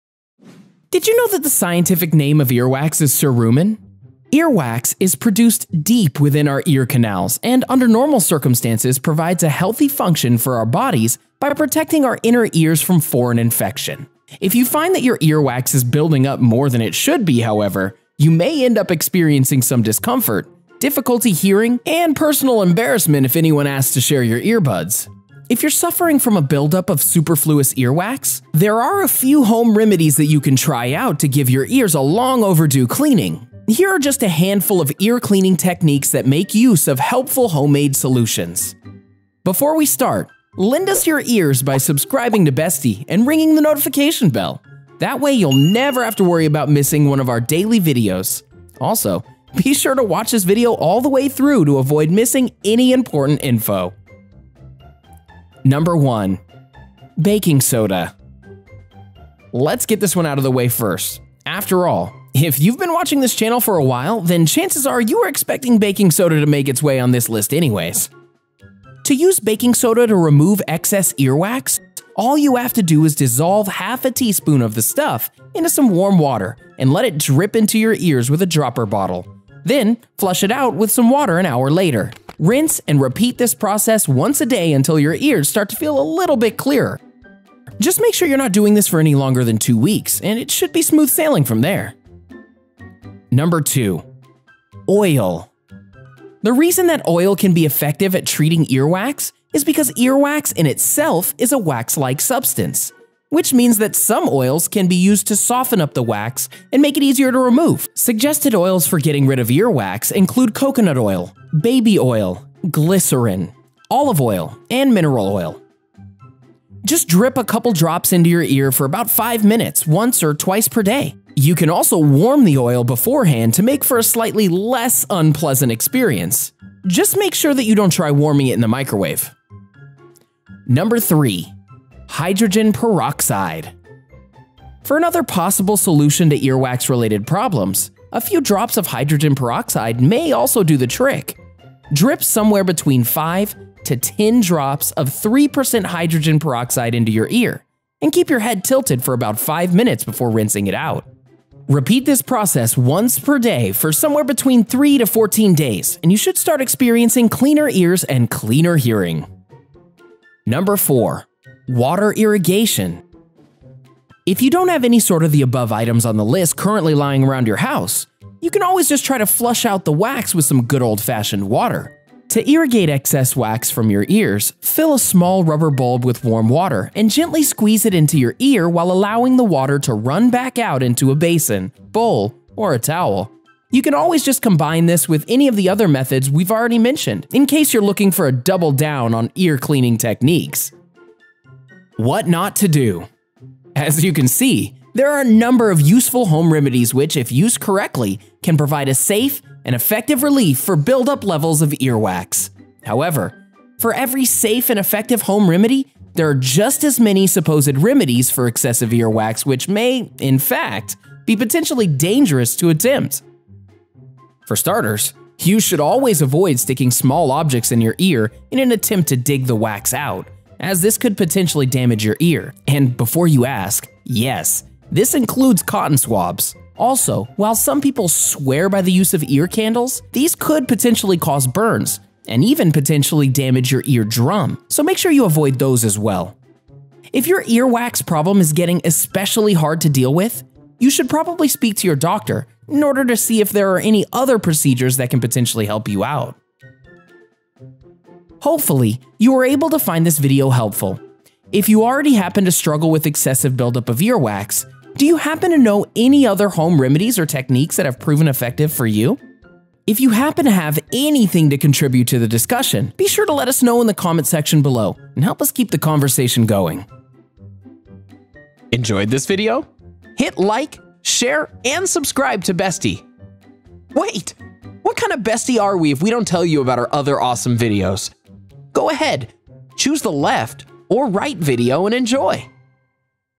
You're Doing It WRONG! - 4 Home Remedies To Remove Ear Wax The Right Way
4 home remedies for removing ear wax. Ear wax is produced deep within our ear canals, and under normal circumstances provides a healthy function for our bodies by protecting our inner ears from foreign infection. But if you notice it's building more than it should, you may experience discomfort, difficulty hearing, and personal embarrassment if anyone asks to share your earbuds.

Timestamps:
Intro - 0:00
Baking Soda: 1:06
Oil: 1:43
Hydrogen Peroxide: 2:20
Water Irrigation: 2:51
 What Not To Do: 3:20

Summary:
1. Baking Soda
Dissolve a teaspoon of baking soda into some warm water and let it drip into your ears with a dropper bottle, then flush it out with some water an hour later. Rinse and repeat once a day until your ears start to feel a bit clearer.

2. Oil
Coconut oil, baby oil, glycerin, olive oil and mineral oil are the reccomended oils for getting rid of extra earwax. Just a couple of drops in your ear for about five minutes, once or twice per day.

3. Hydrogen Peroxide
Drip somewhere betwen 5 to 10 drops of 3 percent hydrogen peroxide into your ear, and keep your head tilted for about five mintues before rinsing it out.  Repeat this process once per day for somewhere between 3 to 14 days and you should start experiencing cleaner ears and cleaner hearing.

4. Water Irrigation
To irrigate excess wax from your ears, fill a soft rubber bulb with warm water and gently squeeze it into your ear while allowing the water to run back out into a basin, bowl or towel. You can also combine this with any of the other methods we’ve already mentioned, in case you’re looking to double down on ear cleaning techniques.

For every safe and effective home remedy, there are just as many supposed remedies for excessive ear wax which may, in fact, be potentially dangerous to attempt. For starters, you should always avoid sticking small objects in your year in an attempt to dig the wax out, as this could potentially damage your ear. And before you ask; yes, this includes cotton swabs.